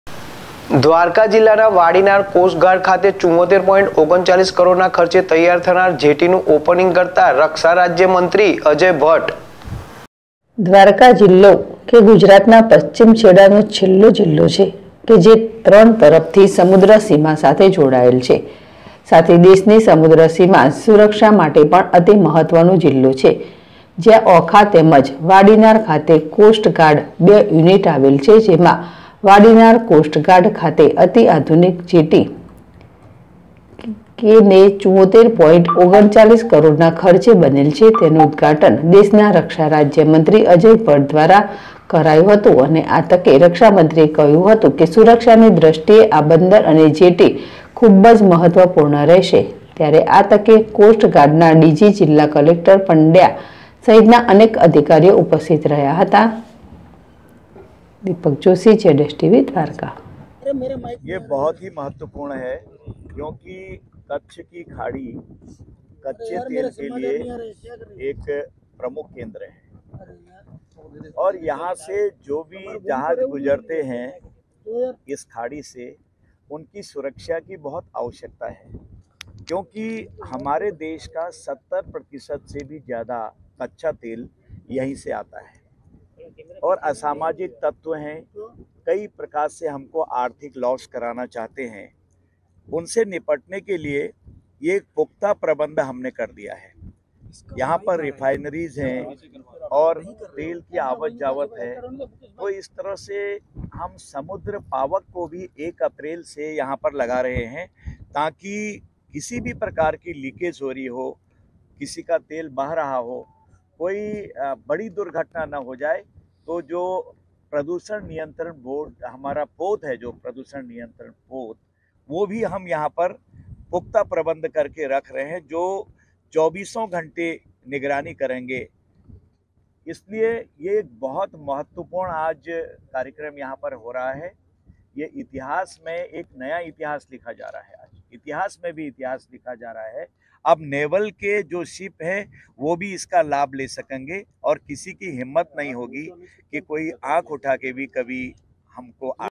દ્વારકા જિલ્લા ના વાડીનાર કોસ્ટ ગાર્ડ ખાતે 74.39 કરોડ ના ખર્ચે તૈયાર થયેલ જેટી નું ઉદઘાટન
દ્વારકા જિલ્લા ના વાડીનાર કોસ્ટ ગાર્ડ ખાતે 74.39 કરોડ ના ખર્ચે તૈયાર થયેલ જેટી નું ઉદઘાટન કરતા રક્ષા રાજ્ય મંત્રી અજય ભટ.